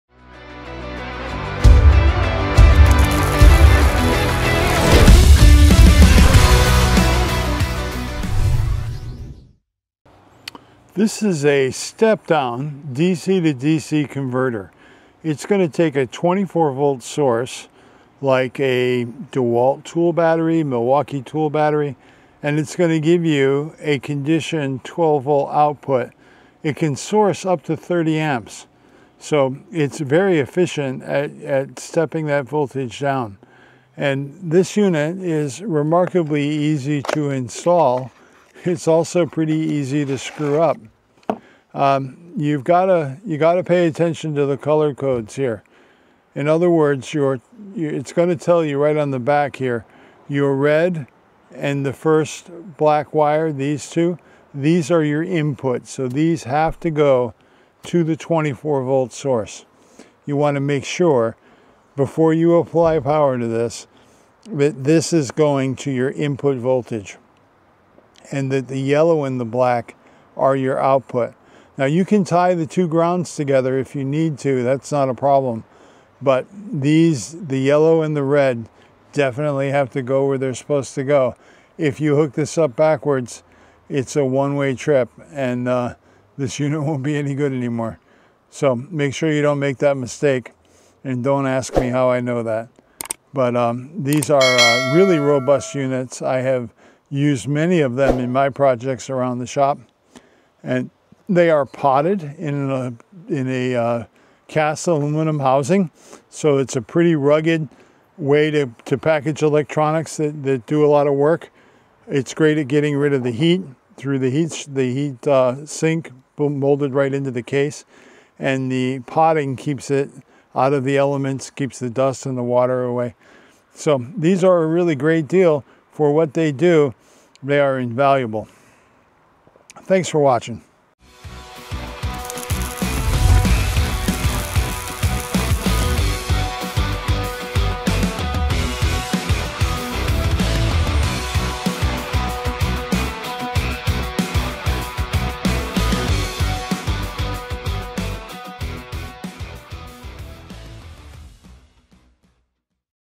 Transform Your Power: 24V to 12V DC Converter Review Essential for Automotive, Marine, RV, & Solar!
In this video, we dive into the powerful and efficient 30 A DC to DC converter that takes a 24VDC input and outputs a stable 12VDC at higher currents. Whether you're upgrading your RV’s electrical system, fine-tuning your marine setup, or enhancing your solar power storage, this converter is a game-changer. With high conversion efficiency (≥93%), robust protections against over-current, overload, and overheating, and a waterproof IP67 rating, this converter is built to perform in the toughest environments.